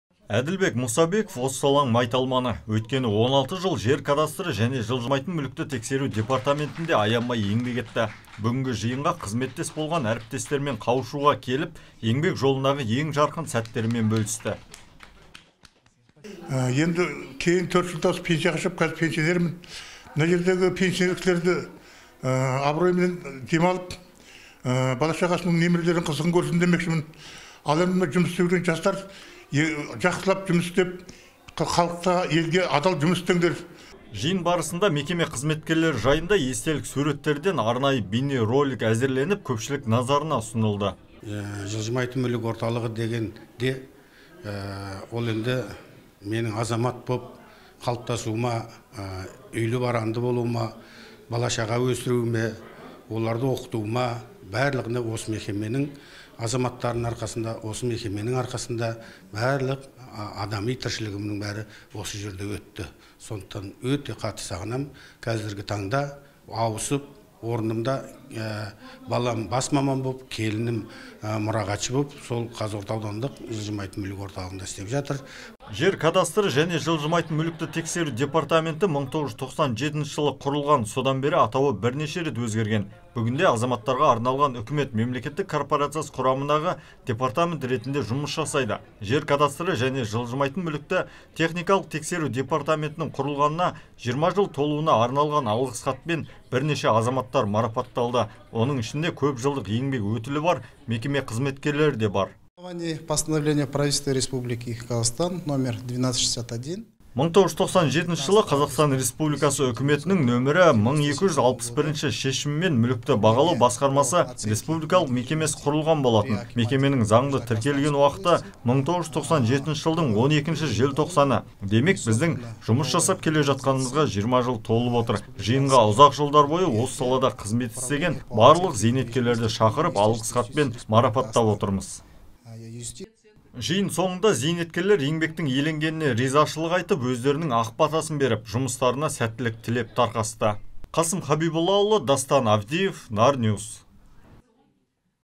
ЖЕР КАДАСТРЫ ЖӘНЕ ЖЫЛЖЫМАЙТЫН МҮЛІКТІ ТЕХНИКАЛЫҚ ТЕКСЕРУ ДЕПАРТАМЕНТІНІҢ ҚҰРЫЛҒАНЫНА – 20 ЖЫЛ!
ЖЕР КАДАСТРЫ ЖӘНЕ ЖЫЛЖЫМАЙТЫН МҮЛІКТІ ТЕХНИКАЛЫҚ ТЕКСЕРУ ДЕПАРТАМЕНТІНІҢ ҚҰРЫЛҒАНЫНА – 20 ЖЫЛ! Мерекеге орай мекеме ұжымы талай жыл осы салада тер төгіп, бүгінде бейнеттің зейнетін көріп отырған азаматтарға құрмет көрсетті.